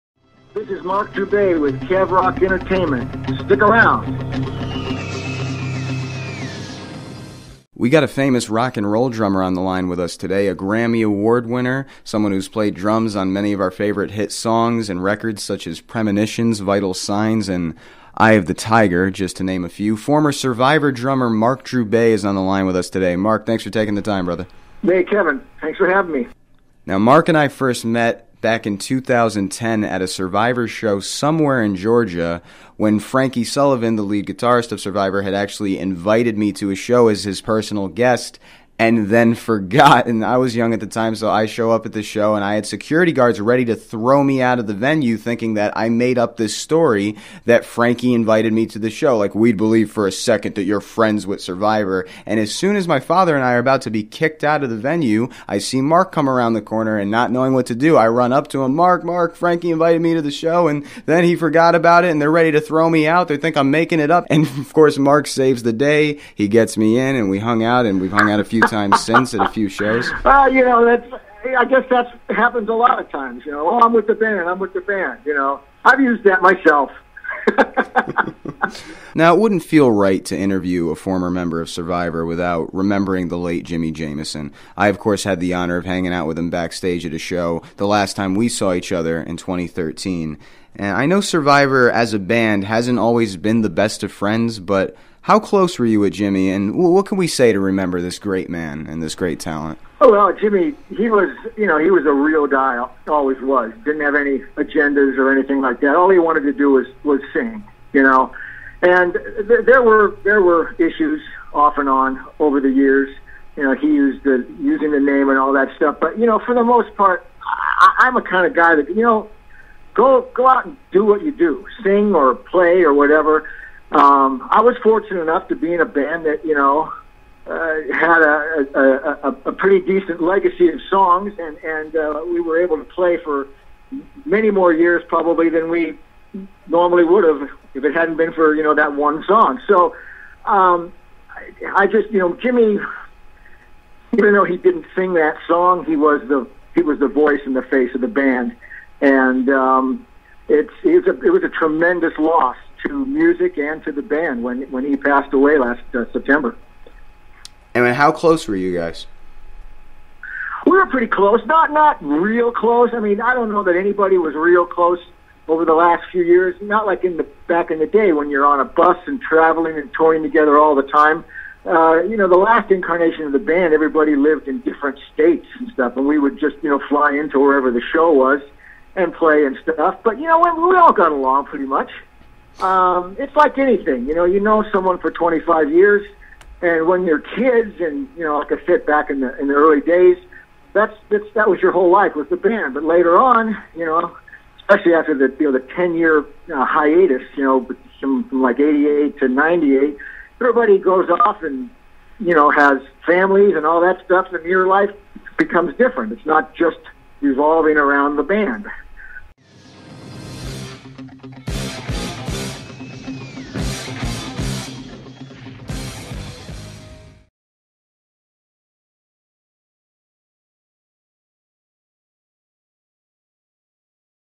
Ex-Survivor Drummer Marc Droubay Remembers Jimi Jamison
Former Survivor Drummer Marc Droubay Remembers front man Jimi Jamison.